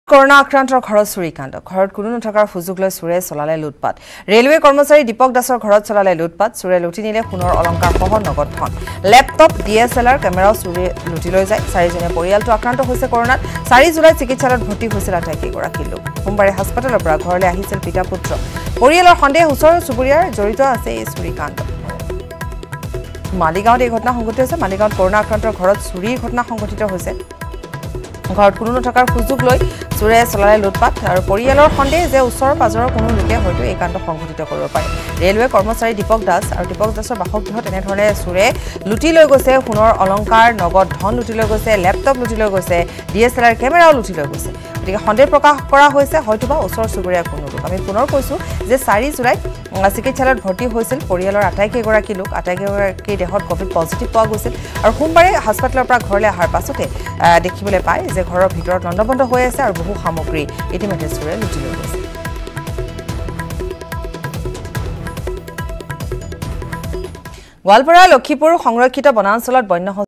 Maligaonত Corona আক্ৰান্তৰ ঘৰত চুৰিকাণ্ড, Laptopকে ধৰি চোৰে লুটি নিলে বহু সামগ্ৰী
ঘৰত কোনো নথকাৰ সুযোগ লৈ চোৰে ঘৰটোত চলালে লুটপাত। ৰে'লৱে কৰ্মচাৰী দীপক দাসৰ ঘৰত চলালে লুটপাত চোৰে।